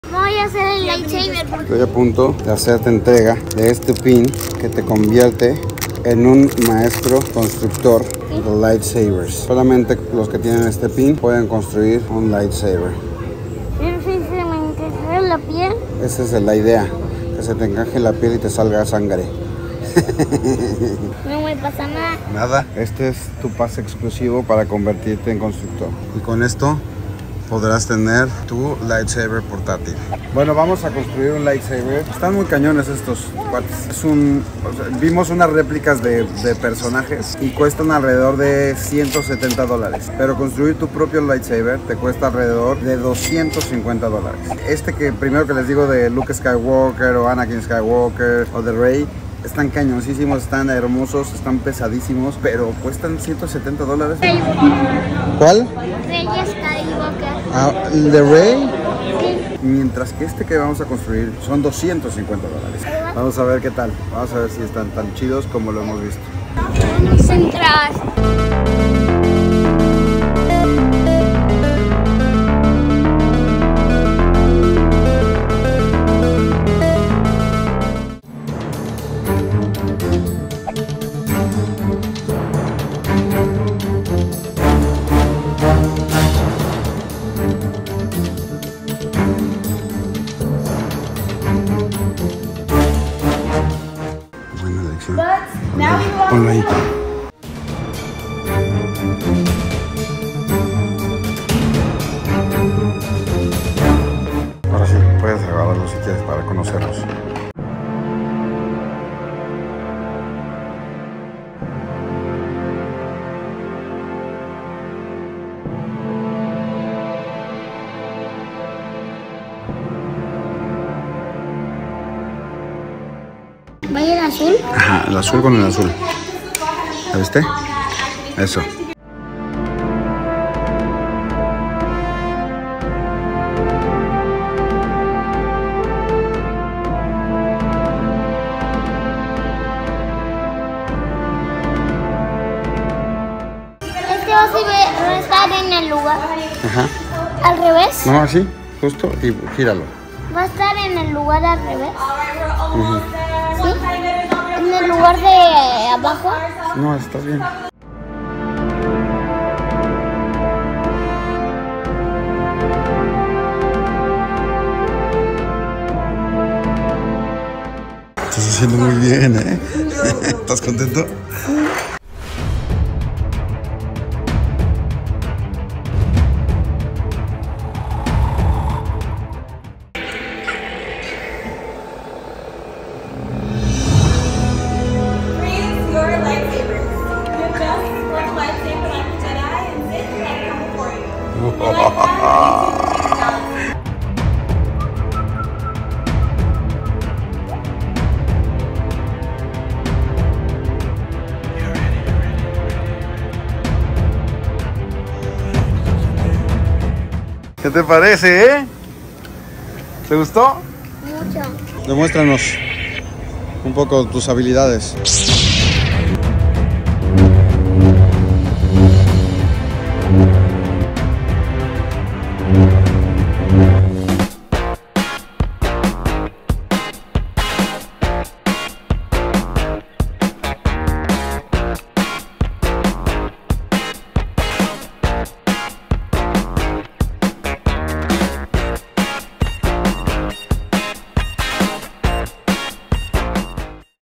Construimos nuestro propio Lightsaber! Esto cuesta hacer el tuyo desde “cero”
Mi hijo se convirtió en un constructor de lightsaber y así vivió esta experiencia.